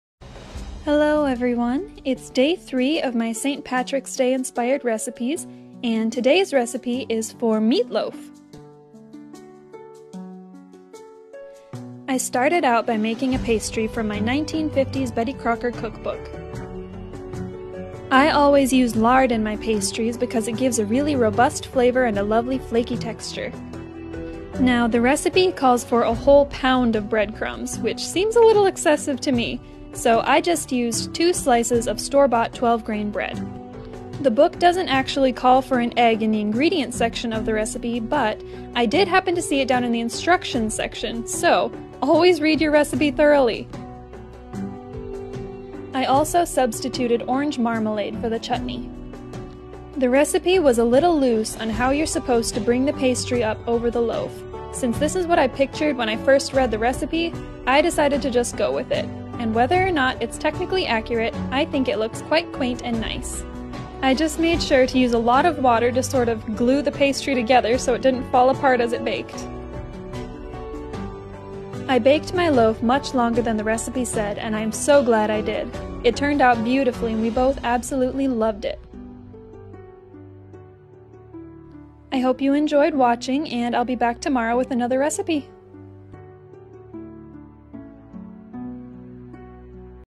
My Week of St. Patrick's Day Themed Recipes--Day3: Irish Meatloaf Recipe
Repost of my March 15th reel, St. Patrick's Day Week, Day 3.

It's day three of my St. Patrick's Day Week recipes! This one is another from my Irish Countryhouse Cooking book, and it's for meatloaf. I've never had herbs in my meatloaf before, or pastry around it, and this was SO good!

MEATLOAF
1 lb meat, minced (I used ground beef)
1 lb Breadcrumbs (I used 2 slices, about 1 1/2 c crumbs)
1 large Onion, chopped
1 T Parsley, chopped
1 T Mint, chopped
1 T Thyme, chopped
1 T Chutney (I used Orange Marmalade)
(1 Egg)
Salt and Pepper to Taste
1/2 lb Shortcrust Pastry

Mix together meat, breadcrumbs, onion and herbs. Add egg, chutney, salt and pepper. Roll out pastry into a square. Pour meat down the centre and trim pastry to come half way over the meat. Damp and cut strips of pastry and lay criss-cross along the top. Bake in moderate oven (350 degrees F) for 1/2 hr. I baked mine for 15 minutes at 425 deg. F, then 35 minutes at 350 degrees F.